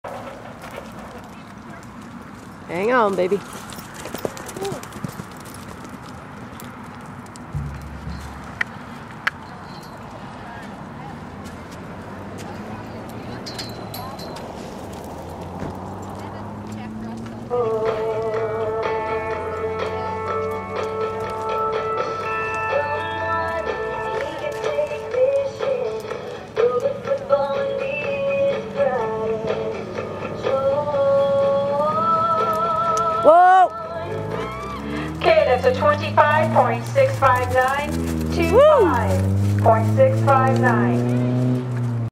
NBHA race 2-14-12.MOD
Missy's 2nd race back after injury